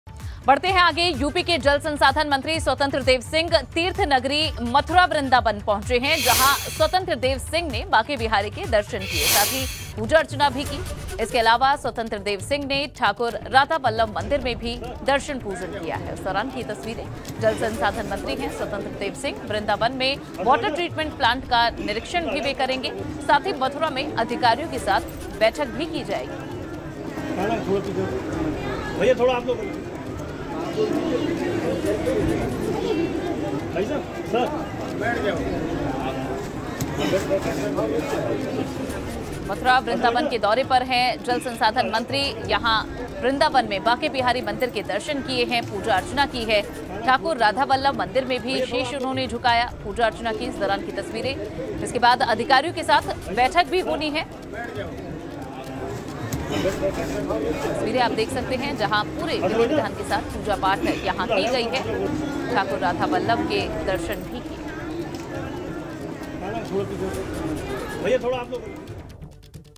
Mathura पहुंचे Swatantra Dev Singh, बांके बिहारी मंदिर में किए दर्शन
यूपी के जल संसाधन मंत्री स्वतंत्र देव सिंह तीर्थ नगरी मथुरा वृंदावन पहुंचे. यहां स्वतंत्र देव सिंह ने बांके बिहारी मंदिर में दर्शन किए. साथ ही पूजा अर्चना की. इसके अलावा स्वतंत्र देव सिंह ने ठाकुर राधावल्लभ मंदिर में भी दर्शन पूजन किया. जल संसाधन मंत्री स्वतंत्र देव सिंह वृंदावन में वाटर ट्रीटमेंट प्लांट का भी निरीक्षण करेंगे. साथ ही मथुरा में अधिकारियों के साथ बैठक भी करेंगे.